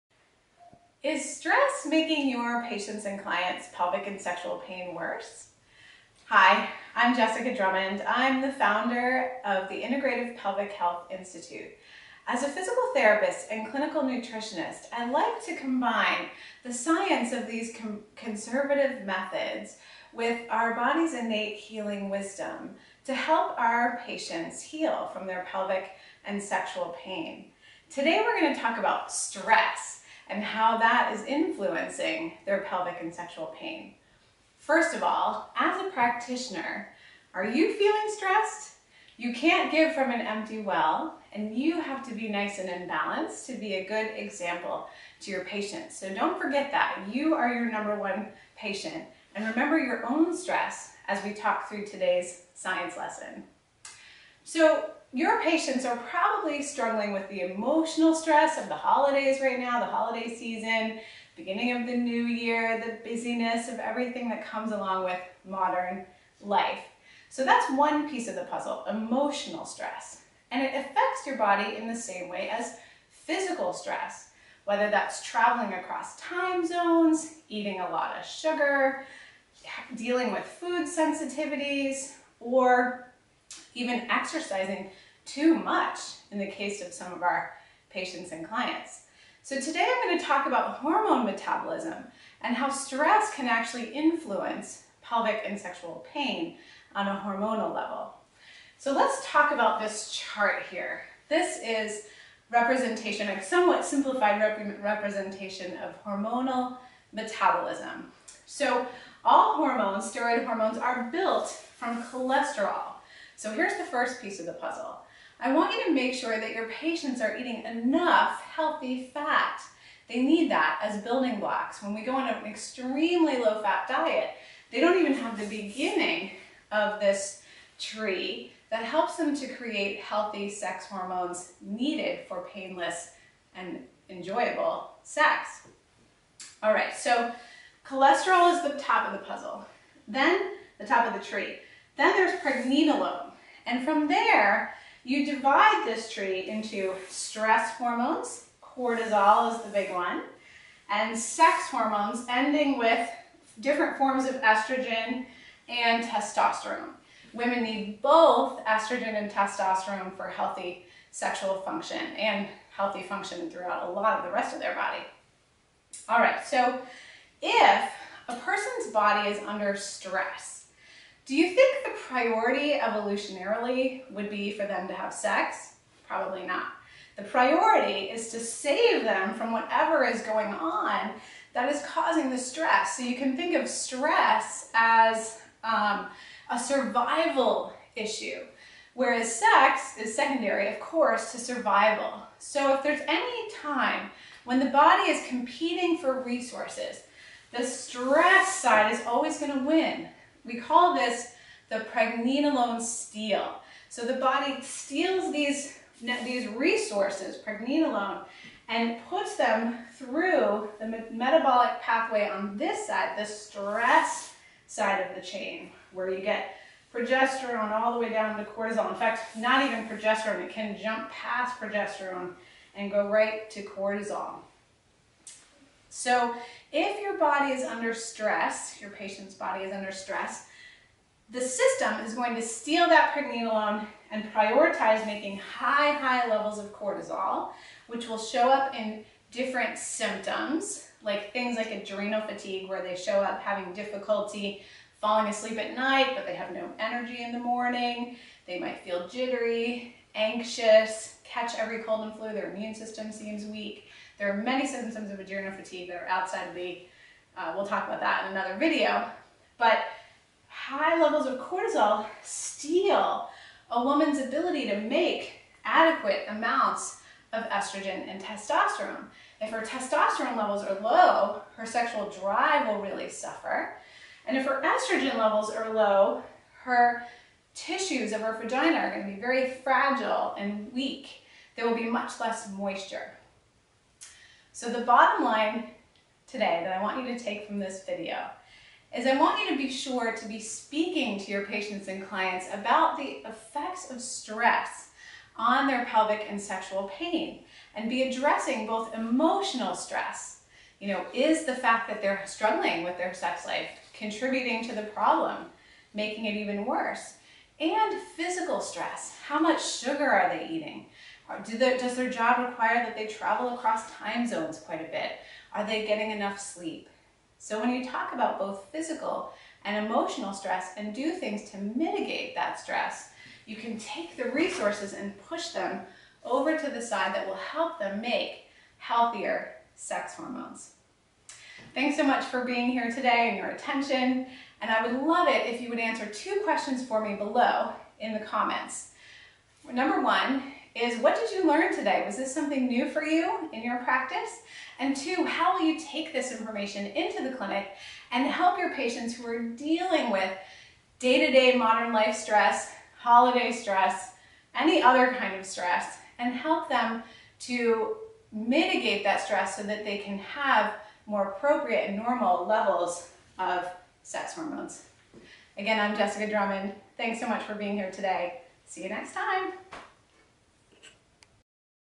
How Stress Impacts Pelvic Pain for People with Endometriosis
How does stress specifically influence sex hormone levels, especially for our clients with endometriosis and other pelvic pain conditions who are under constant stress? Learn more from Dr. Jessica Drummond, DCN, CNS, PT founder of The Integrative Women's Health Institute. Get a free copy of Outsmart Endometriosis by clicking